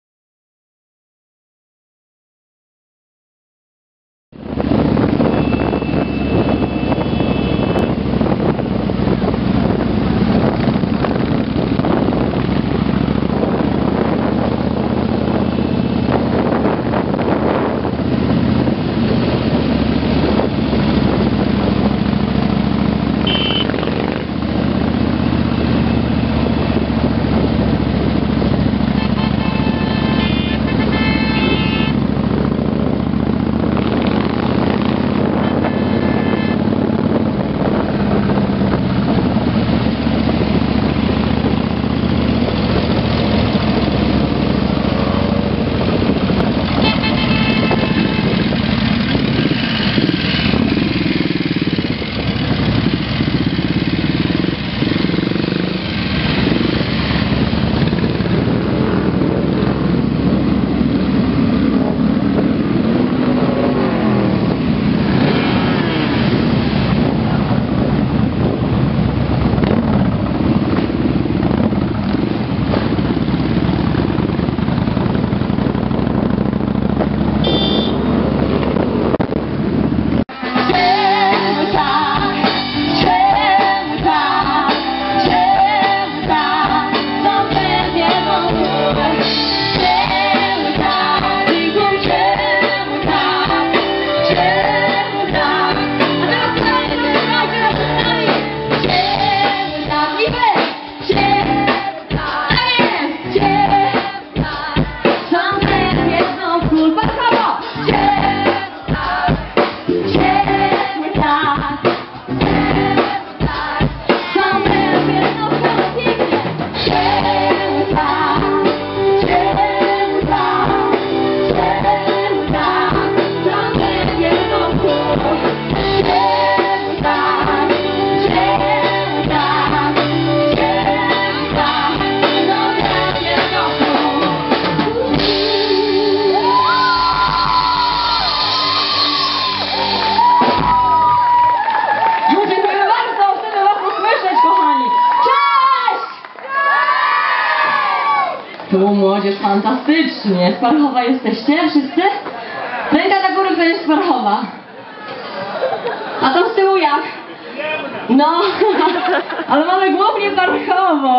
Moto Folk Kaszuby Parchowo 2011
Parada motocykli, Weronika Korthals z zespołem. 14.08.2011 nagranie Nikon D90